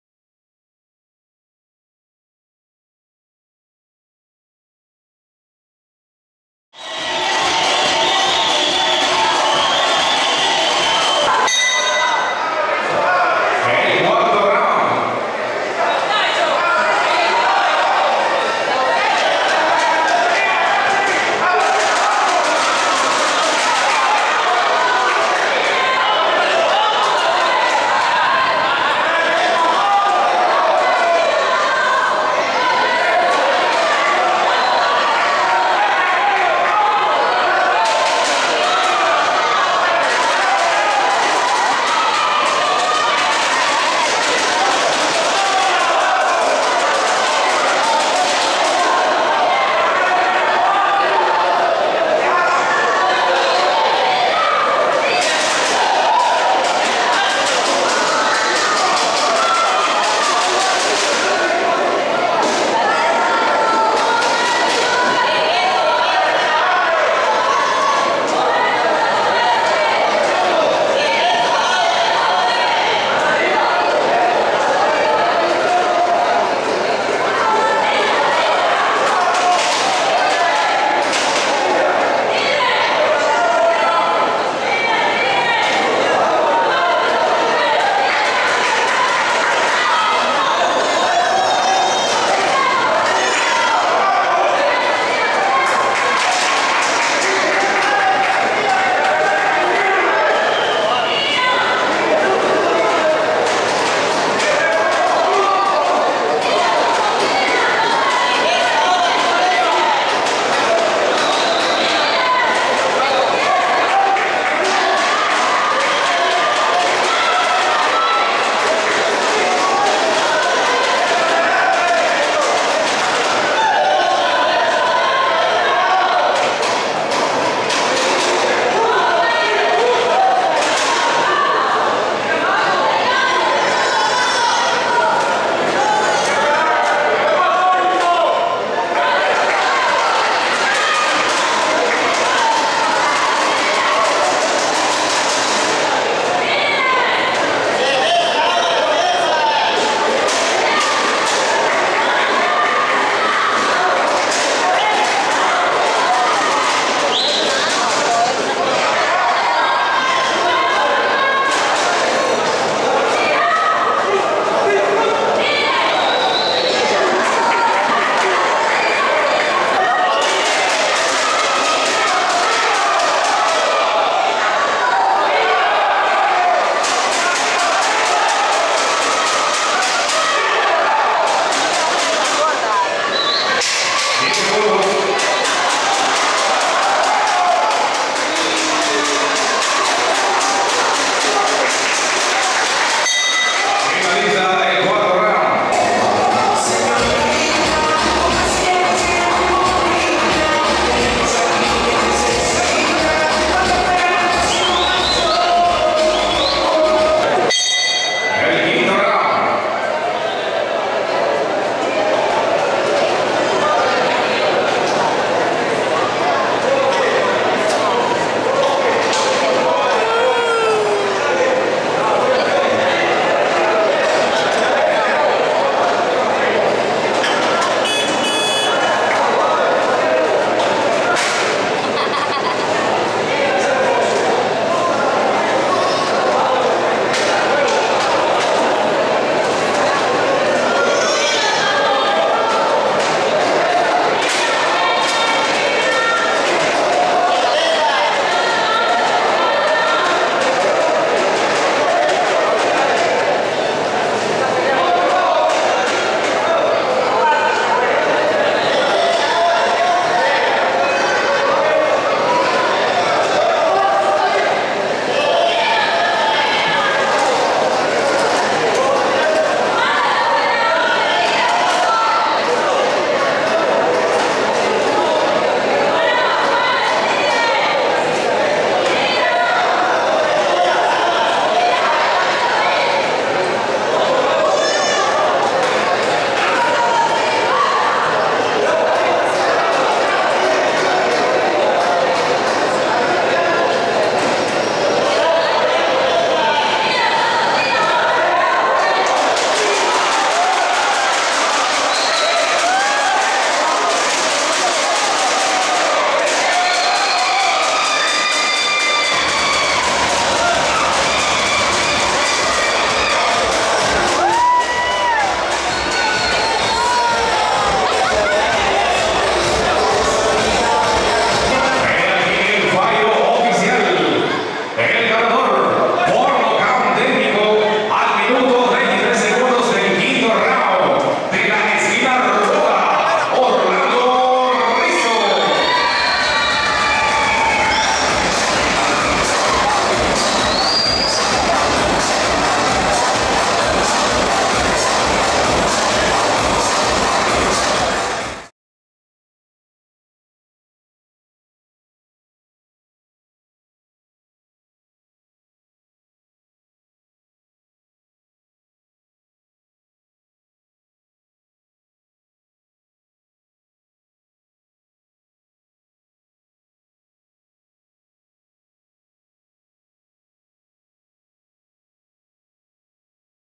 Pelea Orlando Rizo vs Rafael Castillo 2 - parte 2/2
Pelea estelar de la cartelera de la promotora Prodesa correspondiente al mes de Agosto del 2014. En la pelea estelar de la noche Orlando Rizo derrota por TKO en 6 rounds a Rafael Castillo. Version presentado por Prodesa en su pagina web.

Derechos reservado Prodesa y Canal 6.